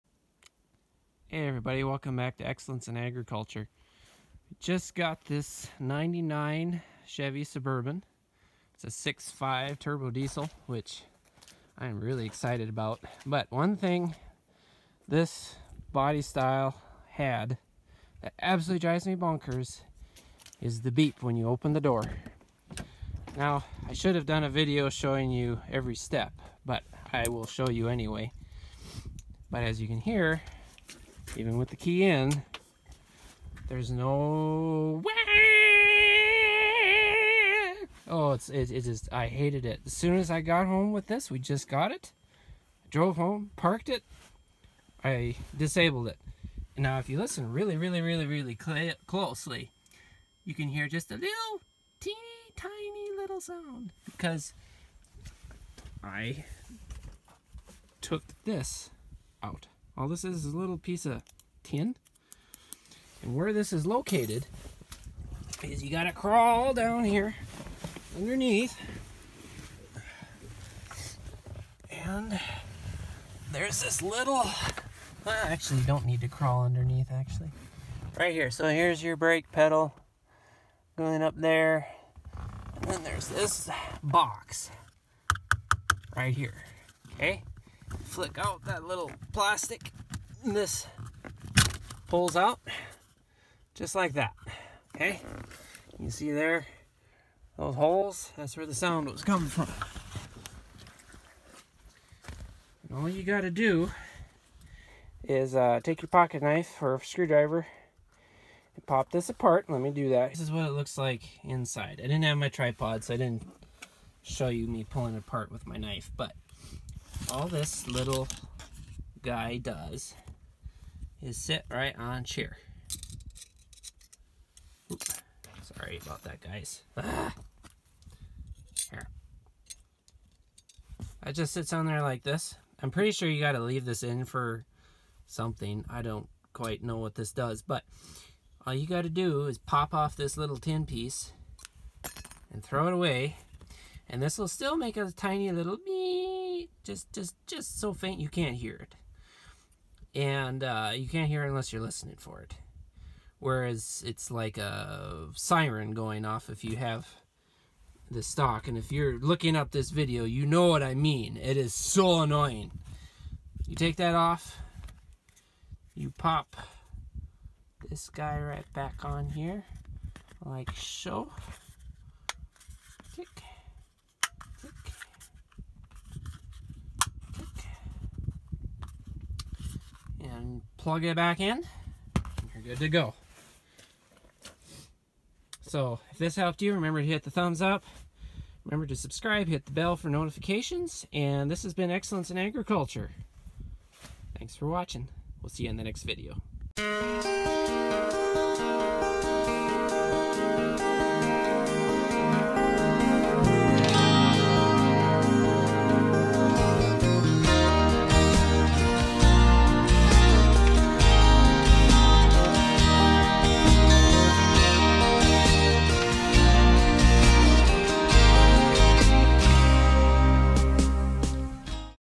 Just make it STOP!!! 99 Suburban Door Buzzer
This easy fix applies to 90's body style GM vehicles, Chevrolet, GMC. For the annoying sound every time you open the door.